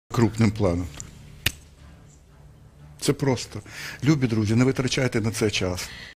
Подвійний фейспалм, не витрачайте на це час | меми українською
Данило Яневський не витримав і зробив подвійний фейспалм. Куди котиться цей світ? Чи є на слідування за ним час? Рукалице -- тягнеться повторити за ним.